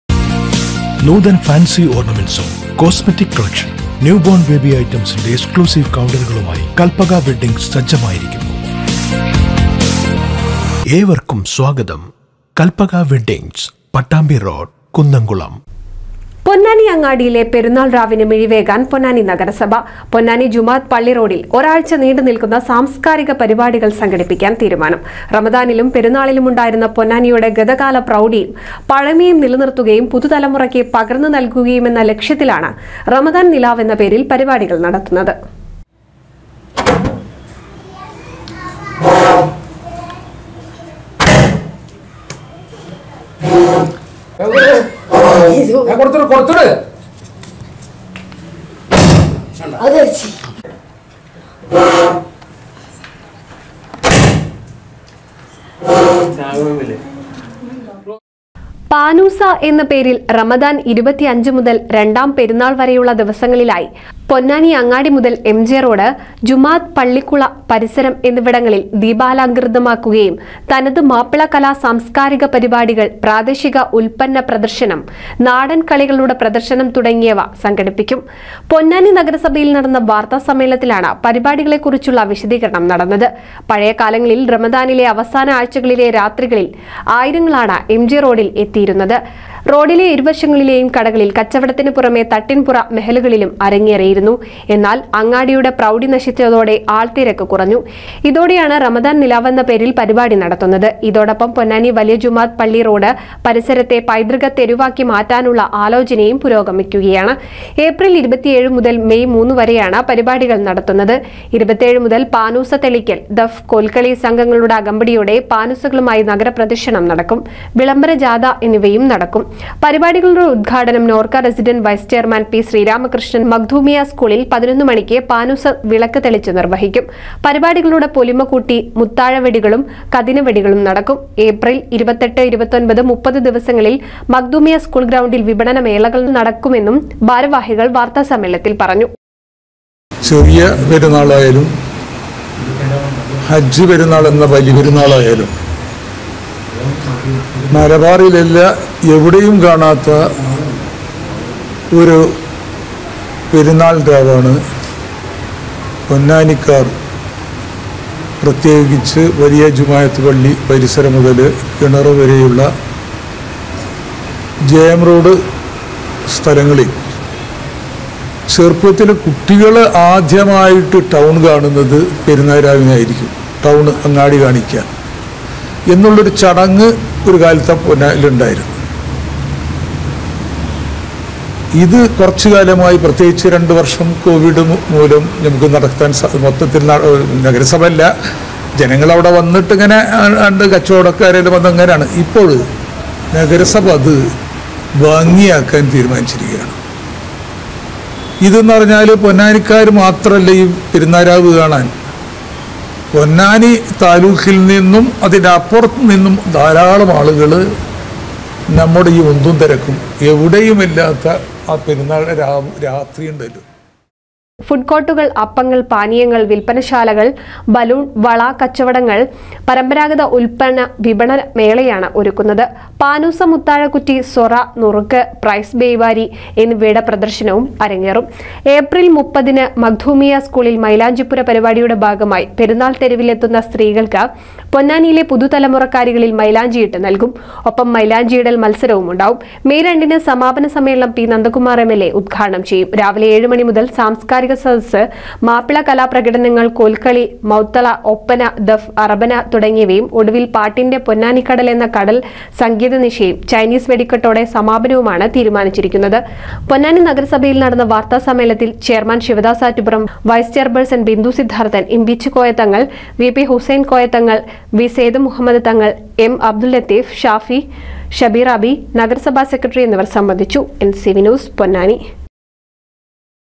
പെരുന്നാള്‍ രാവിനൊരുങ്ങി പൊന്നാനി
പെരുന്നാള്‍ രാവിനൊരുങ്ങി പൊന്നാനി. പൊന്നാനി ജുമുഅത്ത് പള്ളി റോഡില്‍ ഒരാഴ്ച നീണ്ടു നില്‍ക്കുന്ന സാംസ്‌ക്കാരിക പരിപാടികള്‍ സംഘടിപ്പിക്കാന്‍ തീരുമാനം